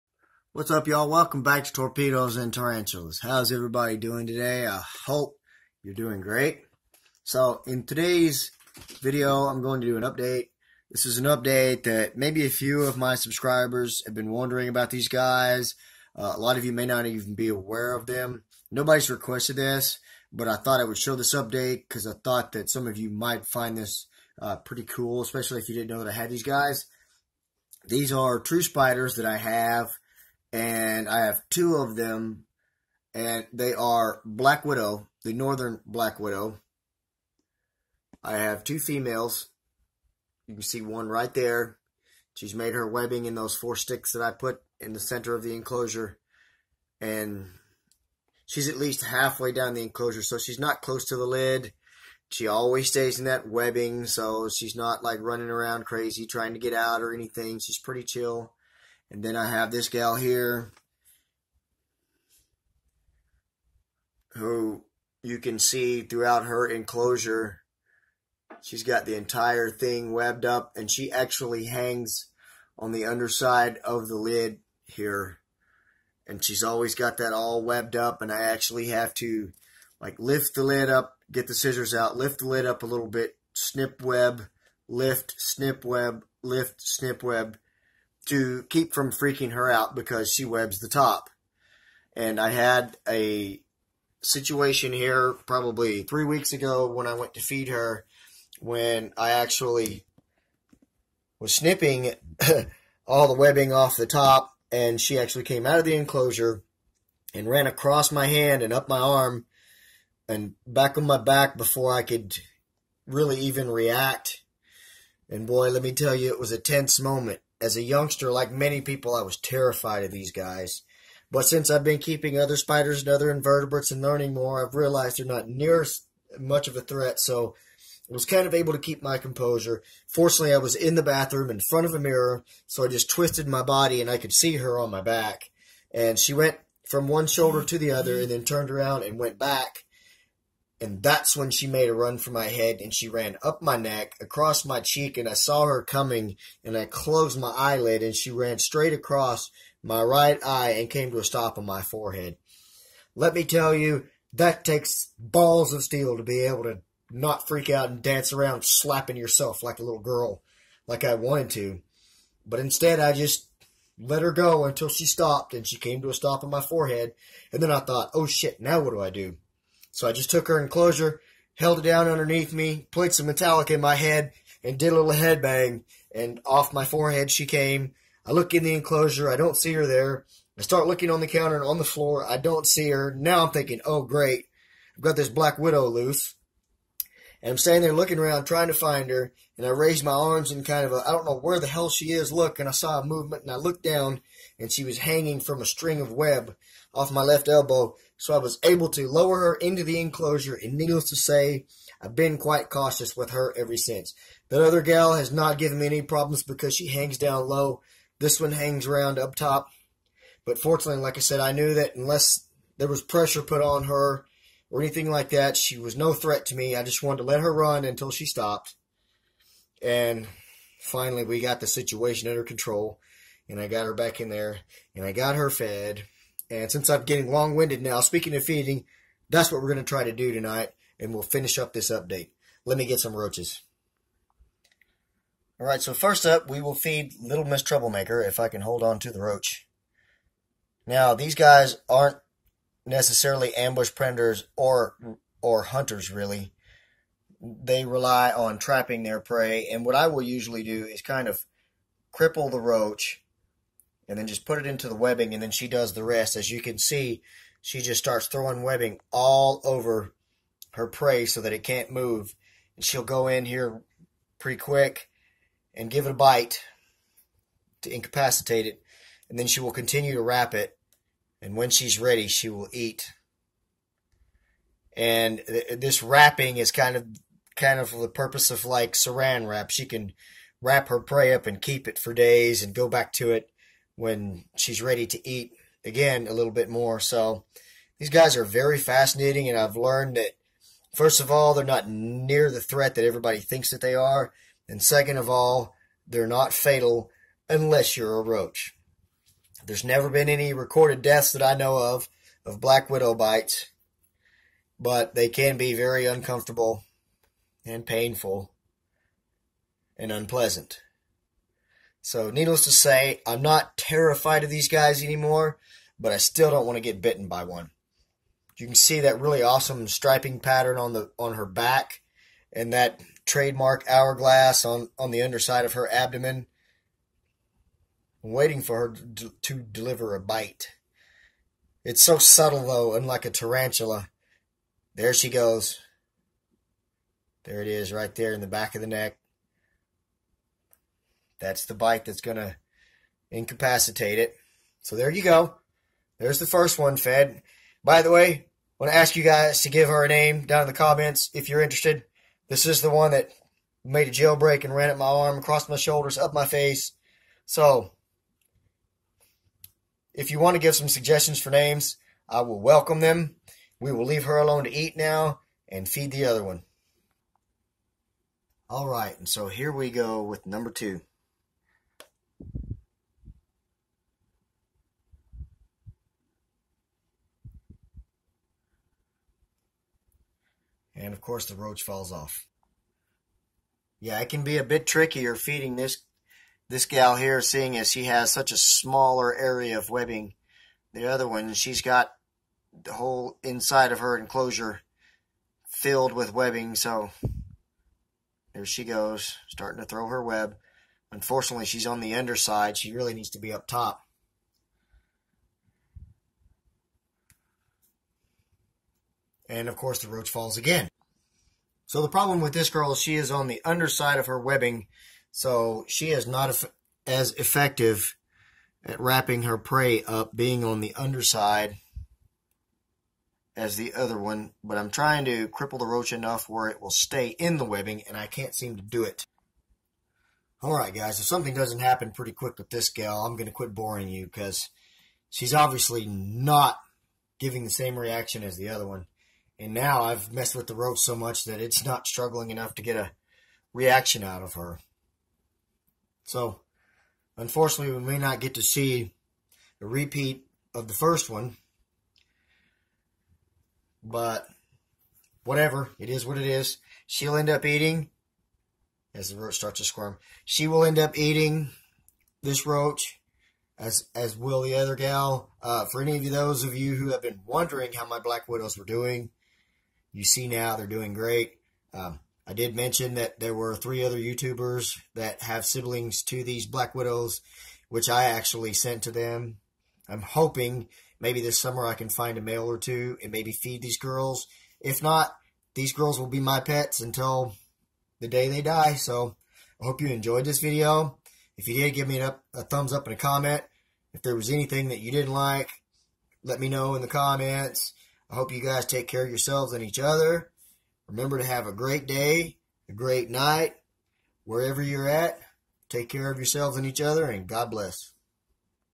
Black widow update. Take care of yourselves & each other 🤣
An update on my two female Northern Black Widows.